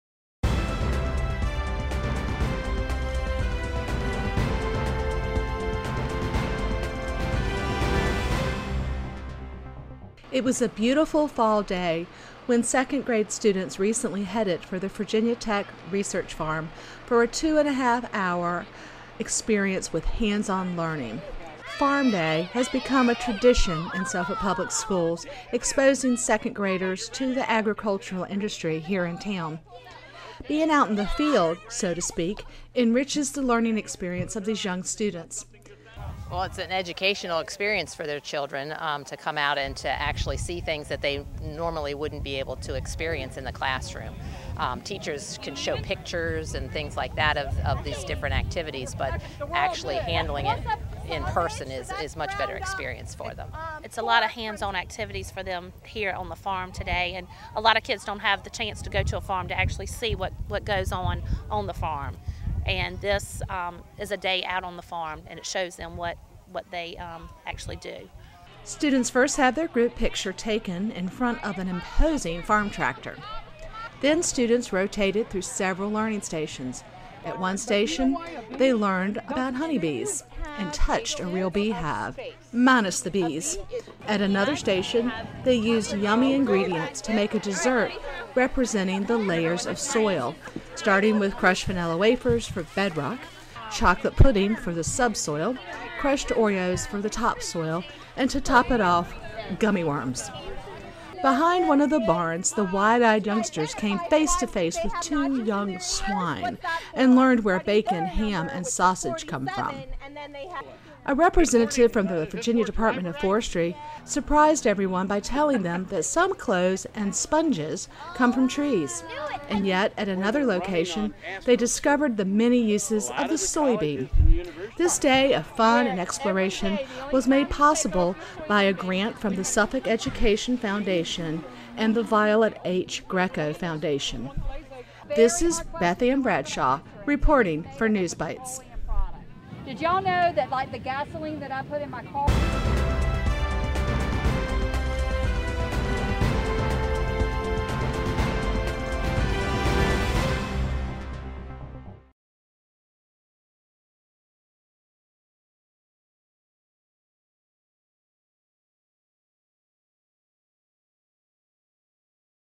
A Day on the Farm
Second grade students learn about swine, soy beans, bees and soil conservation on a real farm.